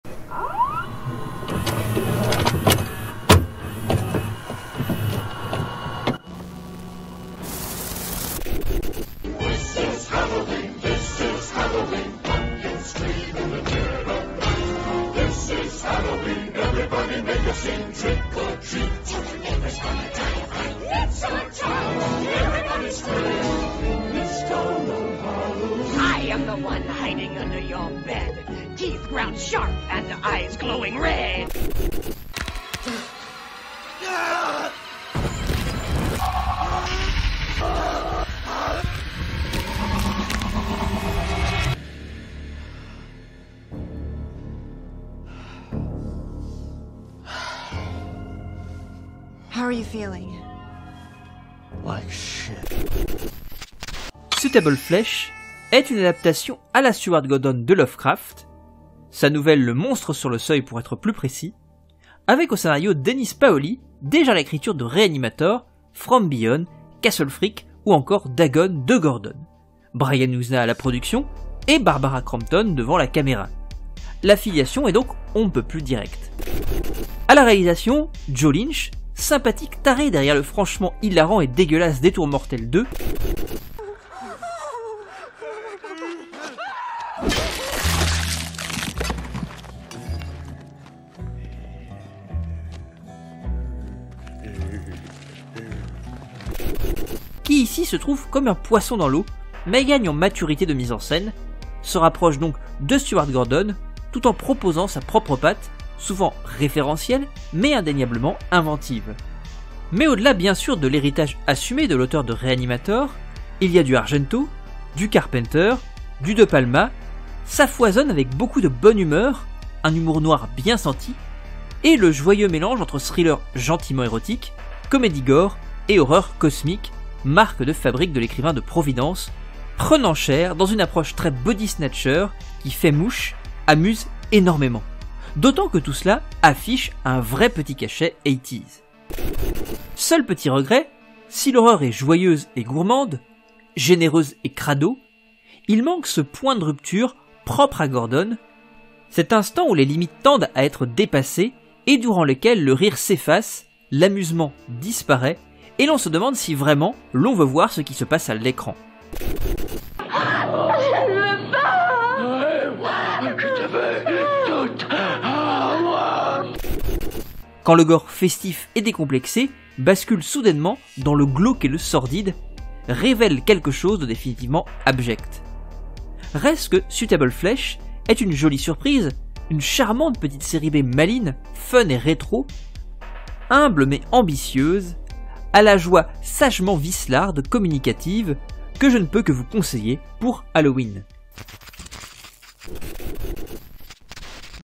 [Ciné] Road to 31 ! ... ou mini calendrier de l'avent pour halloween2023 ! Épisode 31/31
Ce soir nous continuons notre mini calendrier de l'avent, 31 jours jusqu'à Halloween, 31 films mis en image sur la base de quelques-unes de mes publications sur divers réseaux !

Bonne route !

Des bisous ... et à demain !

0:00 Générique
0:31 Chronique